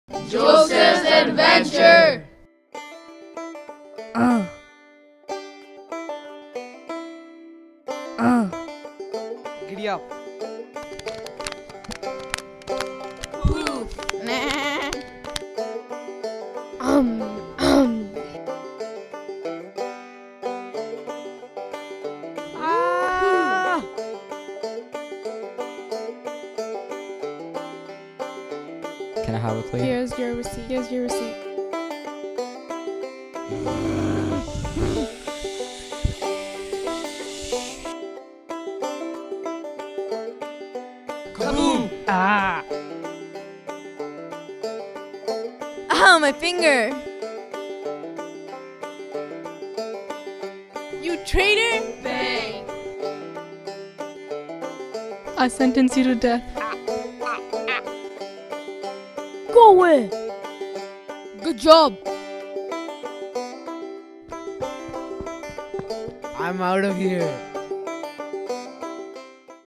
Joseph's Adventure -- Dasmesh School (January 11, 2012)
This short was done by students from Dasmesh School at the Museum of Vancouver in a one-day workshop called Animating History. A short animation demonstration taught them how to do cut out animation. Sound was recorded separately after groups had completed their second scene. Animating History is a partnership of the Museum of Vancouver and the Reel 2 Real International Film Festival for Youth.

About the story:
This story was based on the letters of Joseph Halpenny, a prospector from Gloucester, Ontario that tried his luck at mining in the Cariboo region of BC.